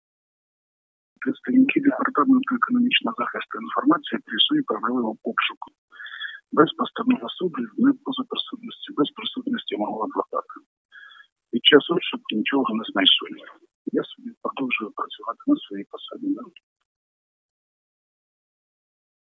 ТНМ — сюжет. У кабінеті голови Кривоозерської РДА відбулись обшуки
У кабінеті голови Кривоозерської РДА  6 лютого відбулись обшуки.   У прокуратурі Миколаївської області нашим журналістам повідомили:
- Первинні слідчі дії досі тривають.
Підтвердив це і сам Олексій Мірошниченко.

Обшуки провели і у інших посадових осіб райдержадміністрації. Правоохоронці обіцяють прокоментувати ситуацію 7 лютого.